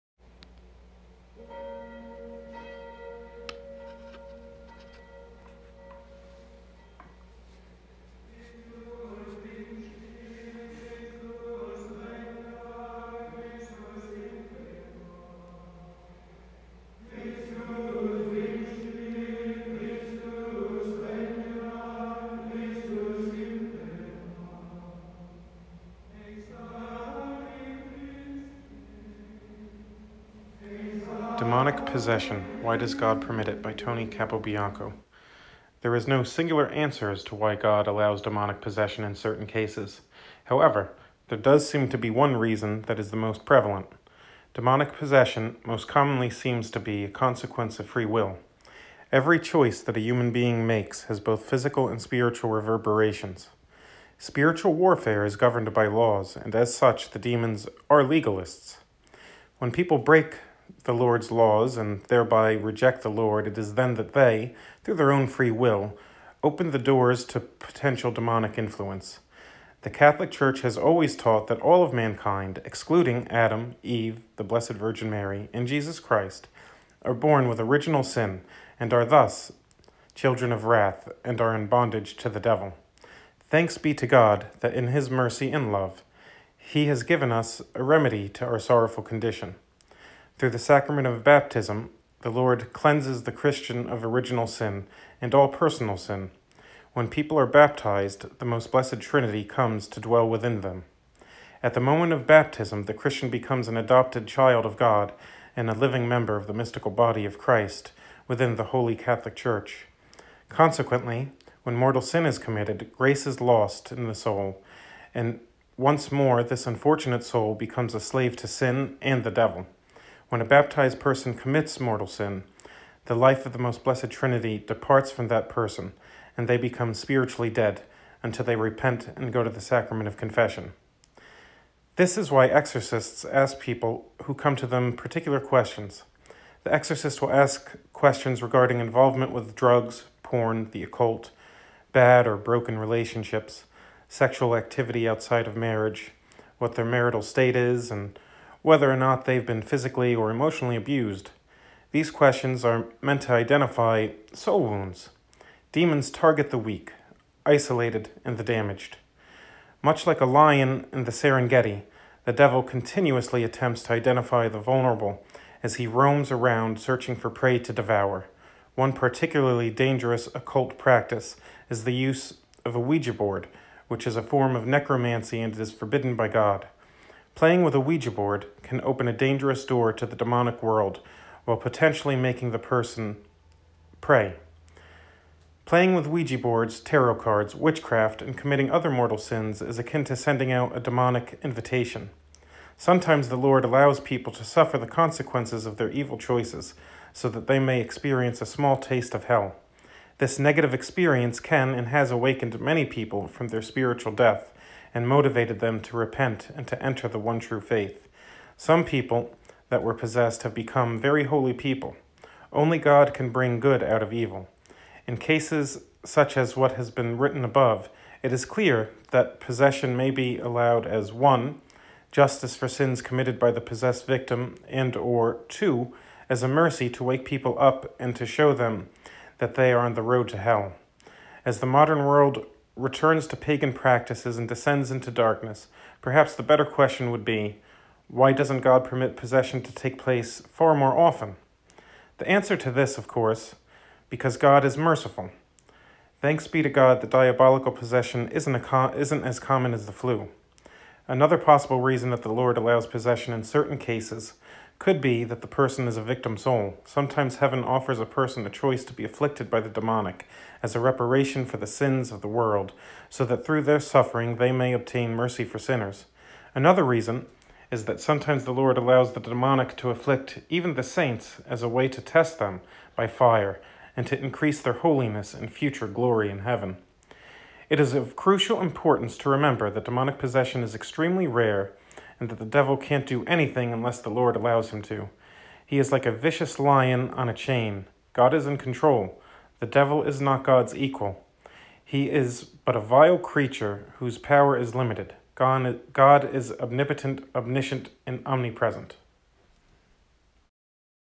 Demonic Possession, Why Does God Allow it?
Traditional Catholic answer to a common question. Why does God allow people to suffer diabolical possession?